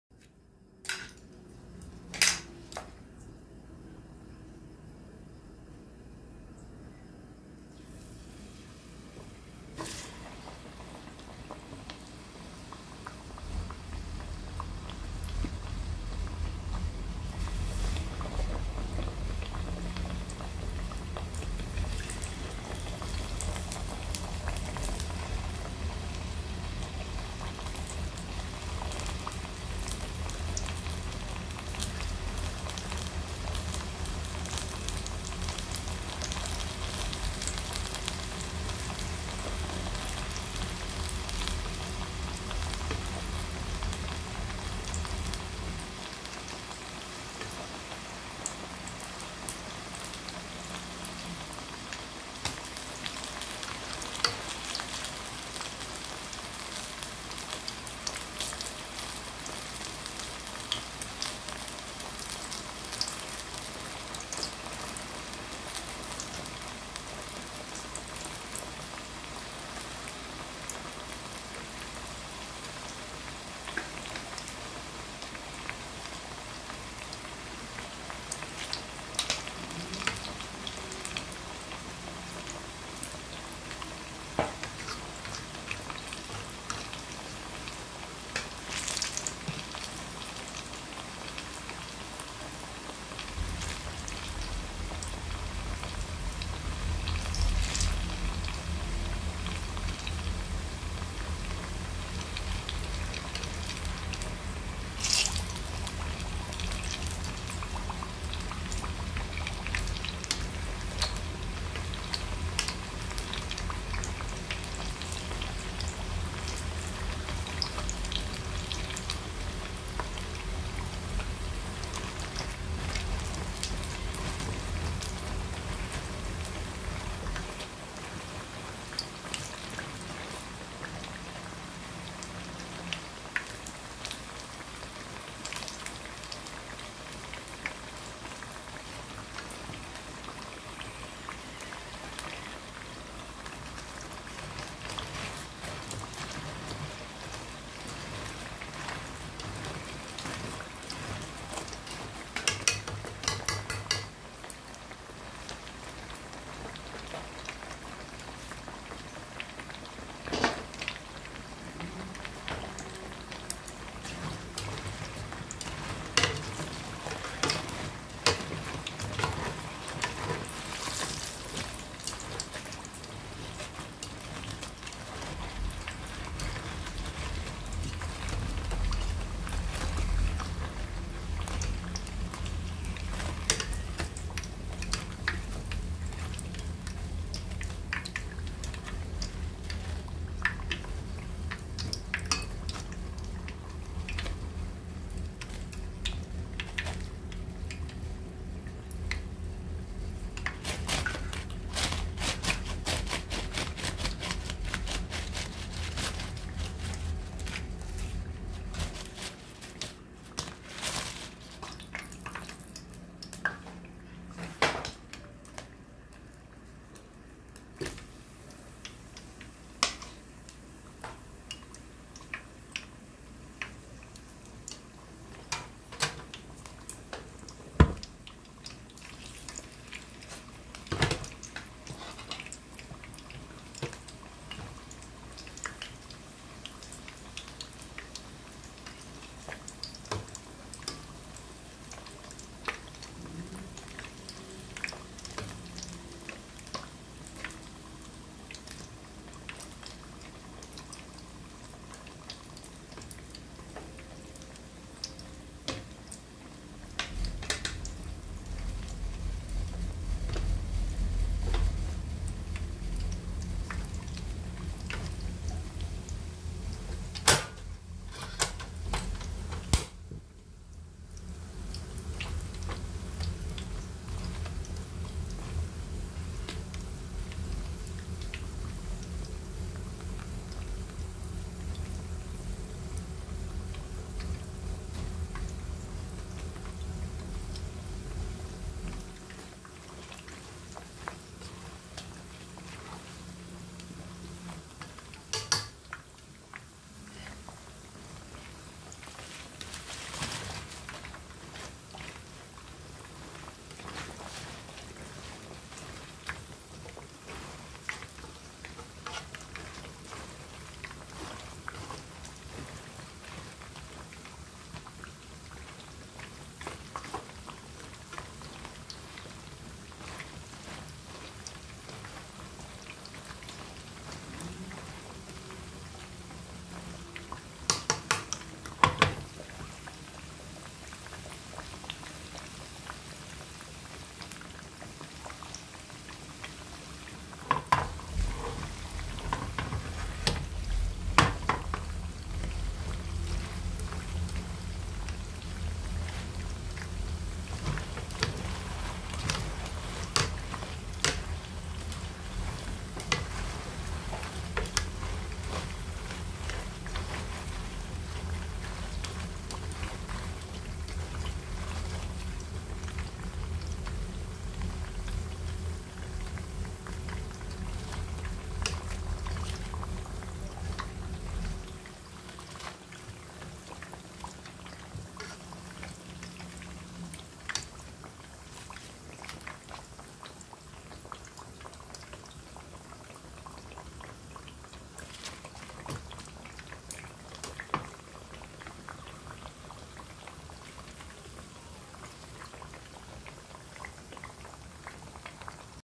새해복많이받으세요#🙏🙏🙏🙇🏻‍♀️
안녕하세요 2022년 새해예요 1월1일토요일저녁이네요 맛깔나는 최여사 만든 고구마튀김 드세요 사랑하는 유친님 부족한데 항상 따뜻한 응원 진심으로 감사합니다 우정입니다🙇🏻‍♀️